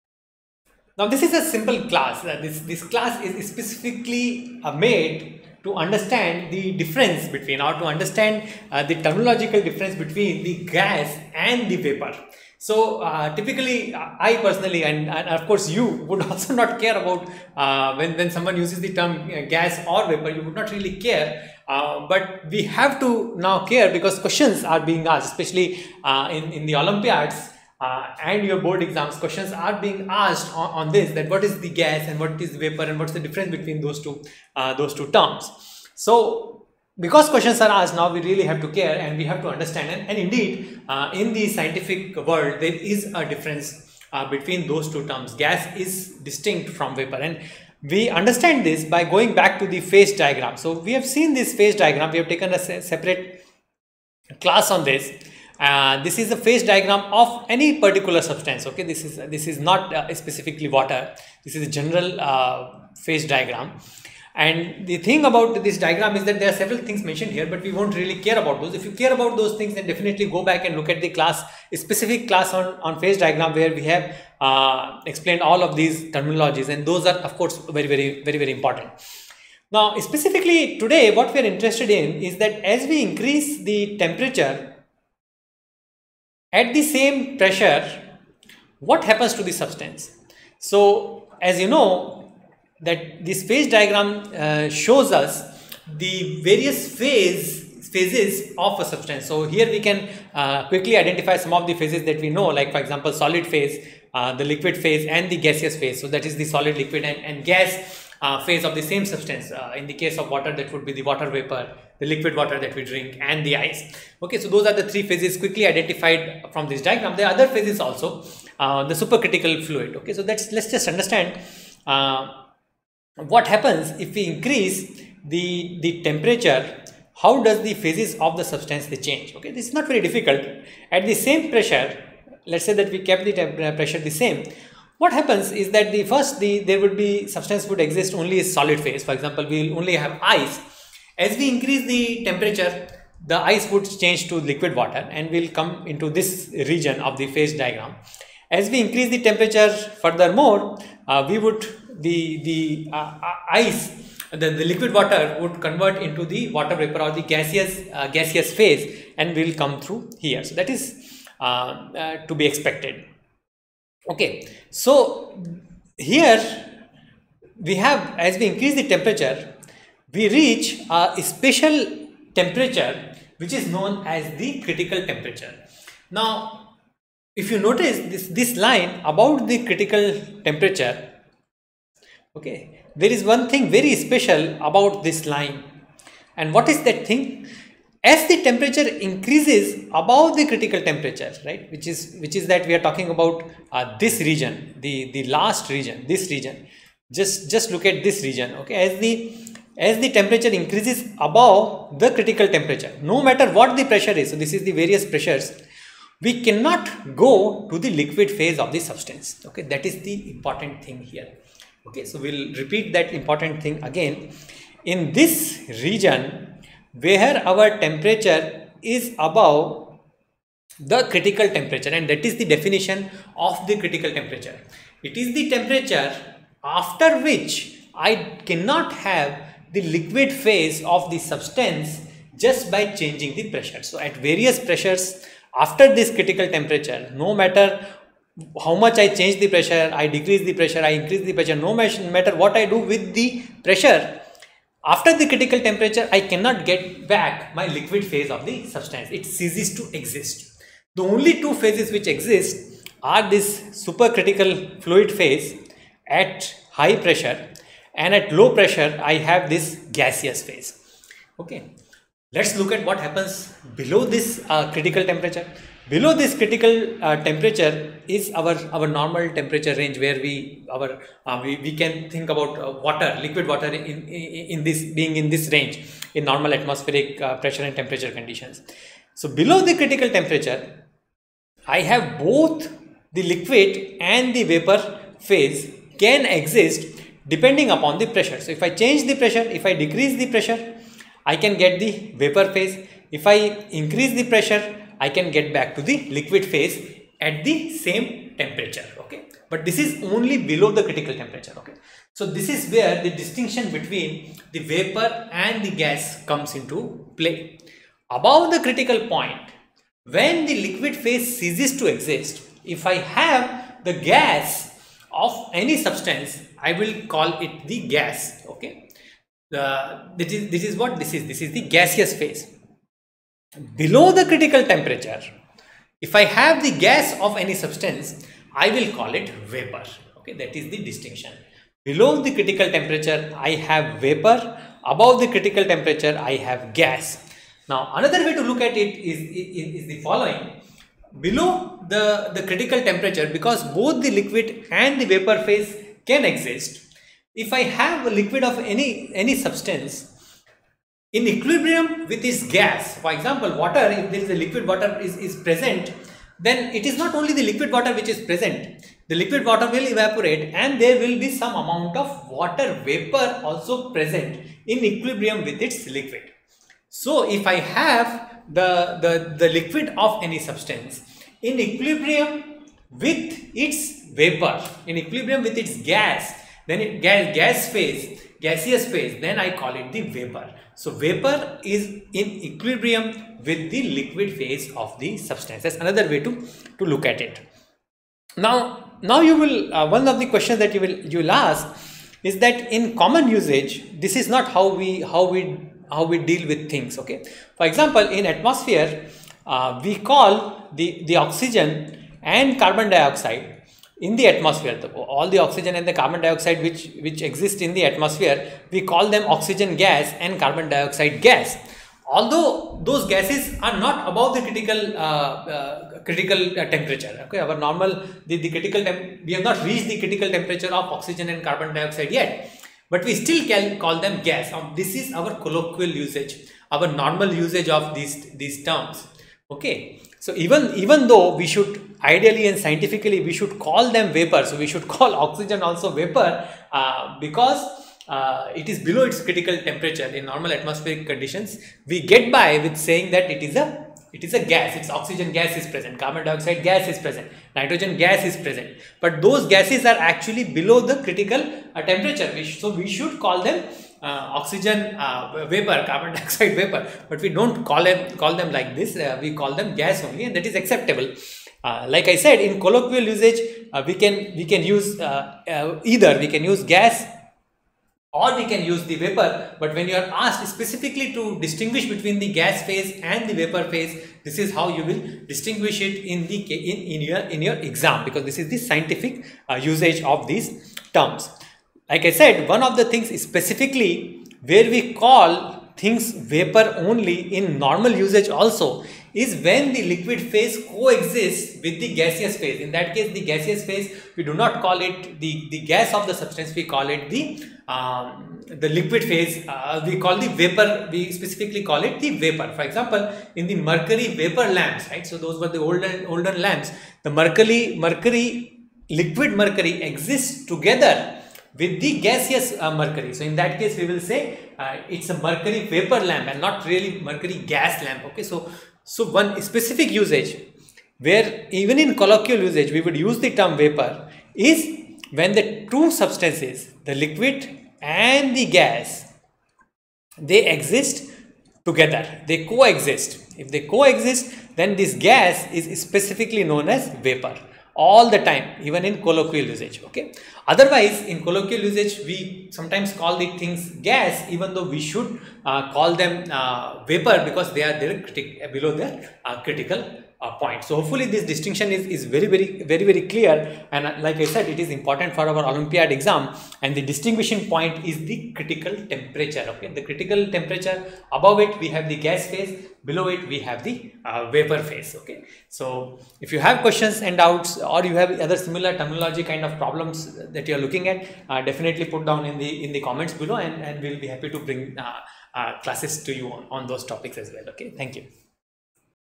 Difference Between Gas and Vapor | What is Critical Temperature | NSO | Olympiad | Gas vs. Vapour
Also understand Critical point- critical point and critical temperature.